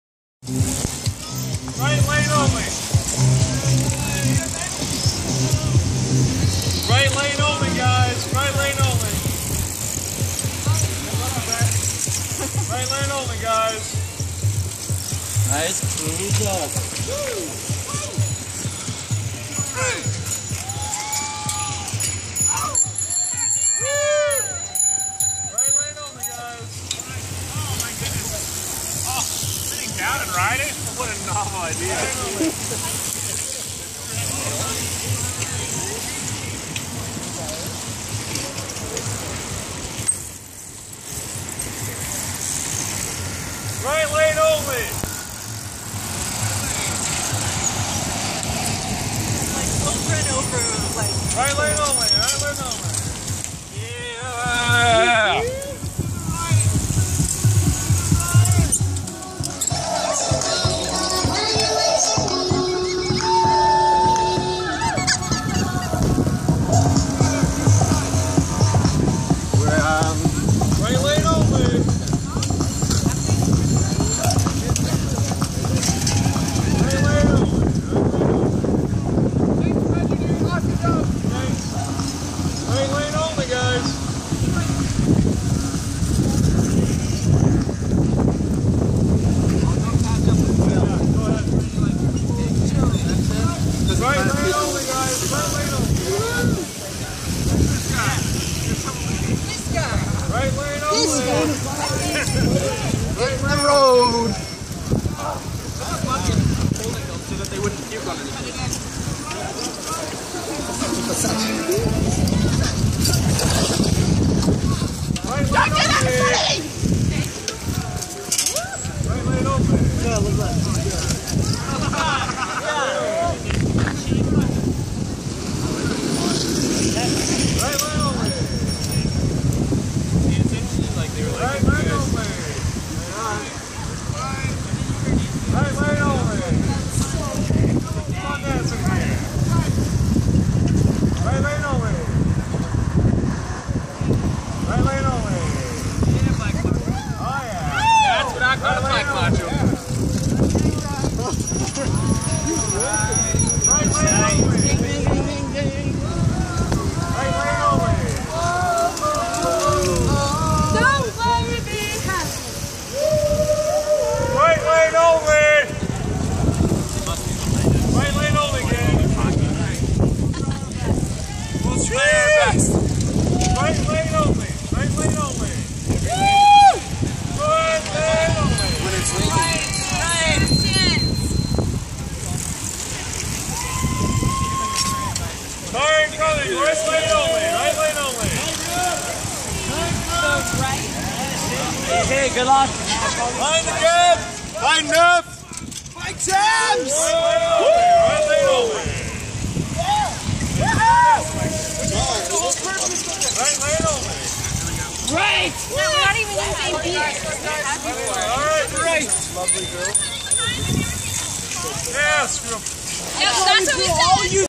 Rainbow Trout Music Festival Bike Jam
RTMFBIKEJAM. it was towards the end of the night but still a rediculous turn out. Good on you Winnipeg